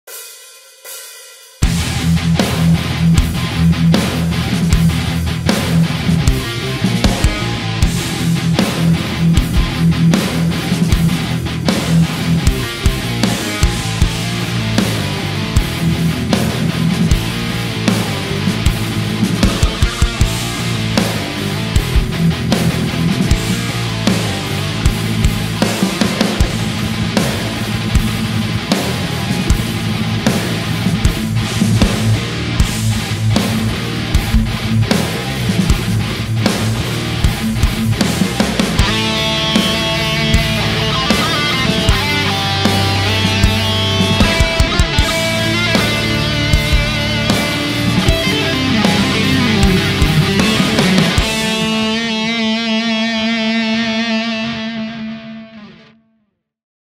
Sunday With Ola #30 - Riff Challenge
Thought i'd challenge myself to try write some riffs.

//Gear Used//
ESP Horizon HRF EMG 57
Kemper EVH 5153 profile
Nolly Architype (DI thickener)